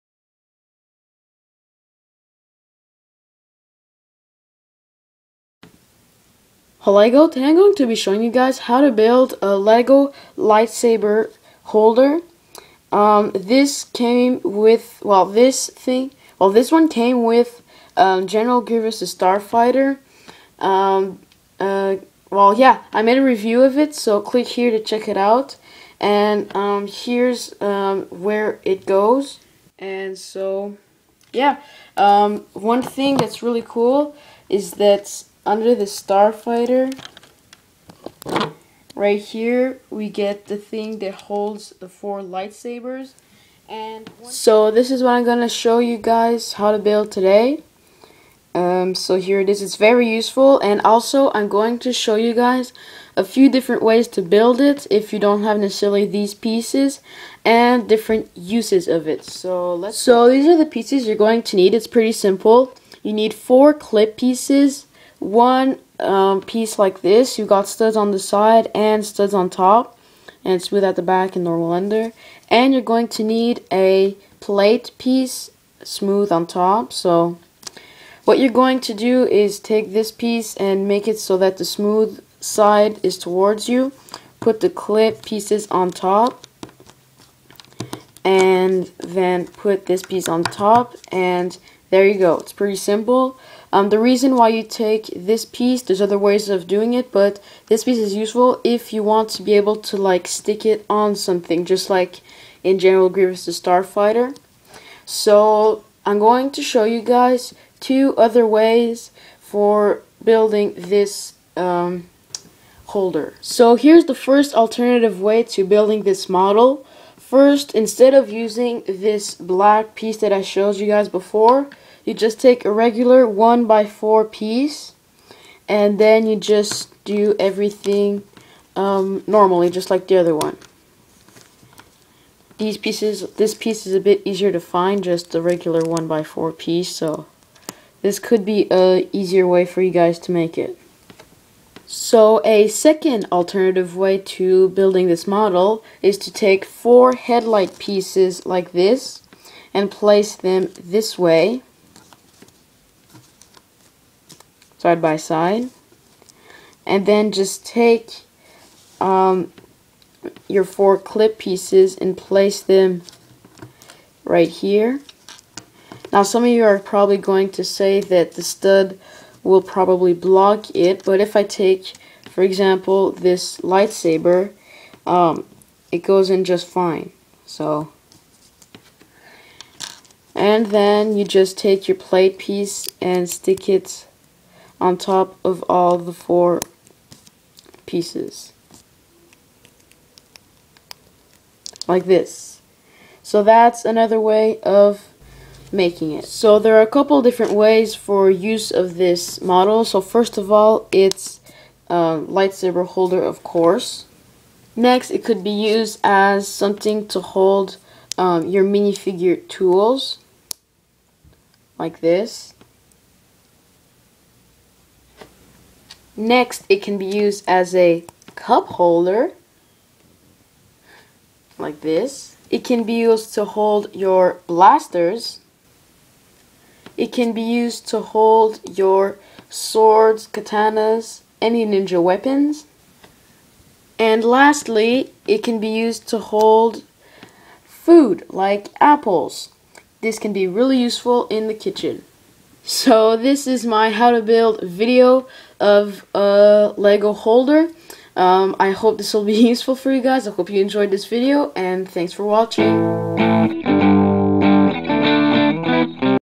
How To Build A Lego Lightsaber Holder/Tool Holder
This video is going to show you how to build a Lego Lightsaber holder!